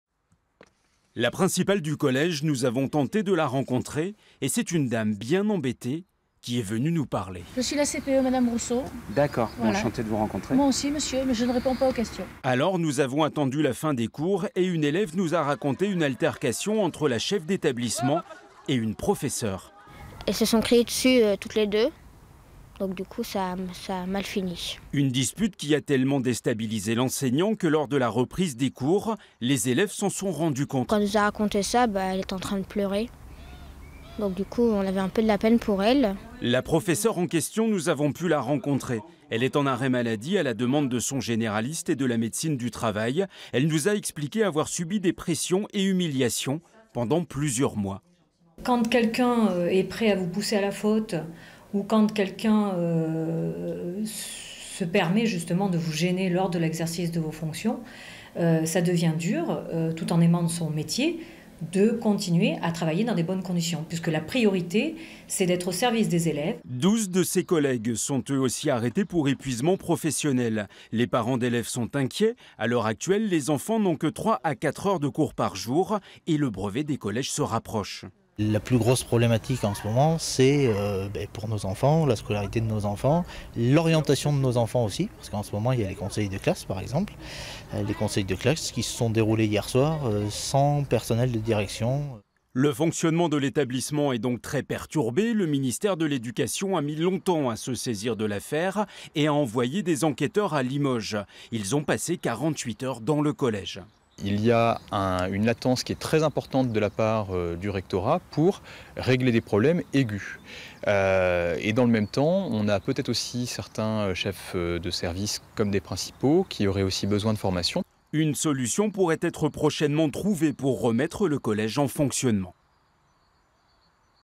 La mal etre des profs au college Firmin Roz
Près de la moitié des enseignants du collège Firmin Roz à Limoges est en arrêt maladie pour épuisement professionnel. Les syndicats parlent d'un problème de management au sein de l'établissement scolaire. Le rectorat assure que la situation sera réglée rapidement.

Reportage Franck Petit, Frédérique Bordes, Marion Haranger

Intervents : Franck Malaurie, Parent d'élève - Thibault Bergeron, Secrétaire académique SE-UNSA